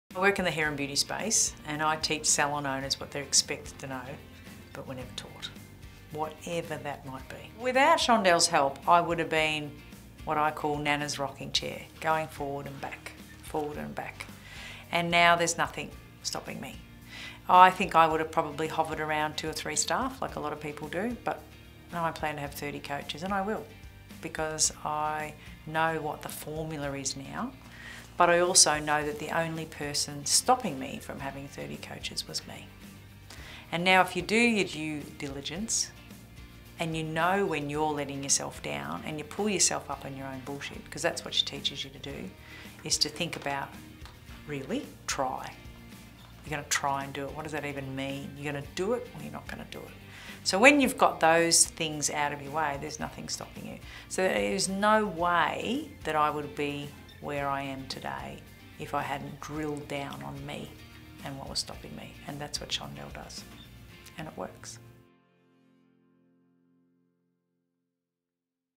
Lisa Conway - Zing Business Coach on Life Puzzle Coaching with Chandell
Profitable Business accelerator alumni talk about the power of the PBA live training day and its impact on their business growth.
You can learn from Chandell as well!

To view her free webinar training and take a business health quiz go